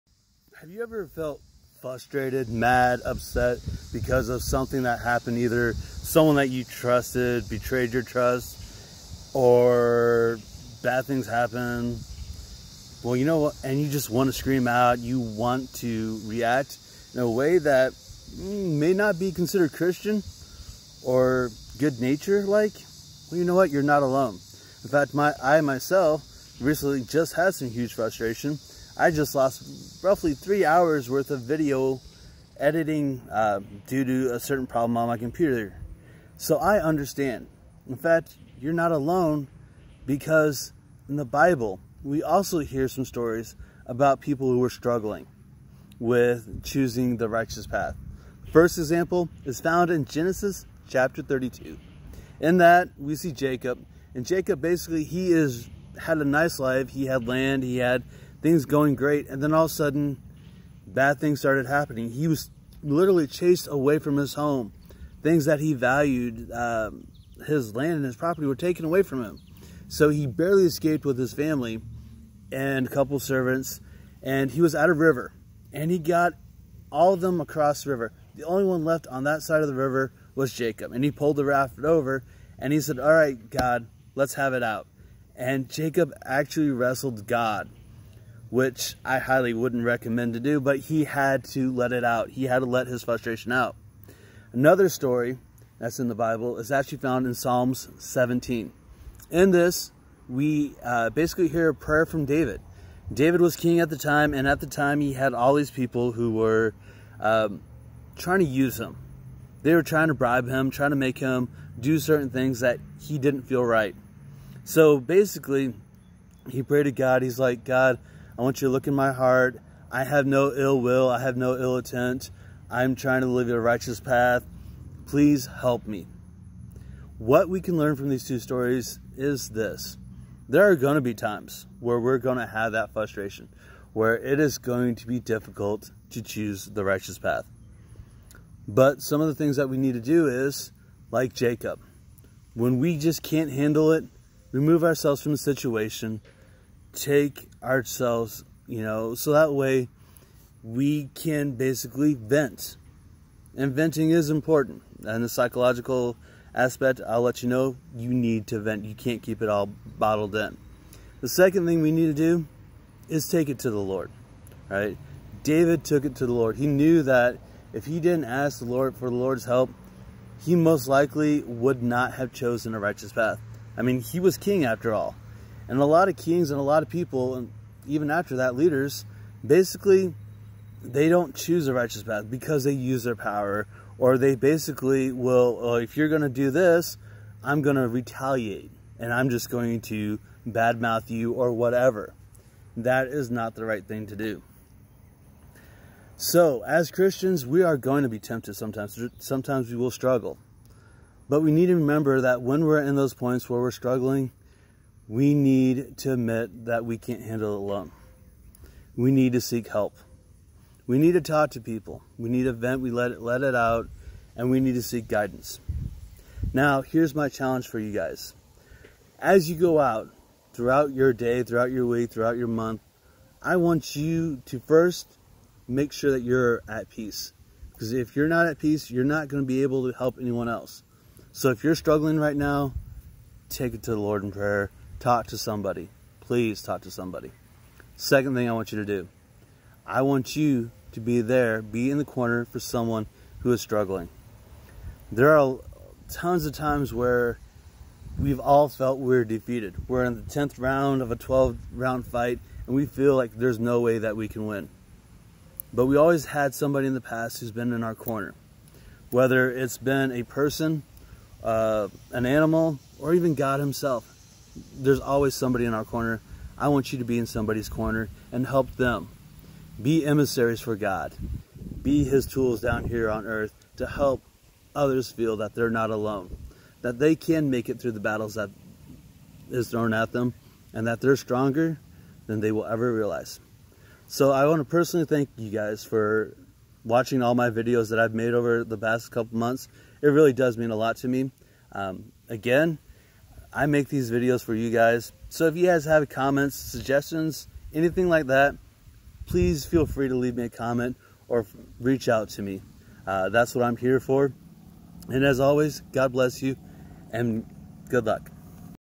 Struggling to do what us right.
Gen. 32 and Psalm 17. We all struggle when life throws us curve balls, don't let that stop you from choosing the right path.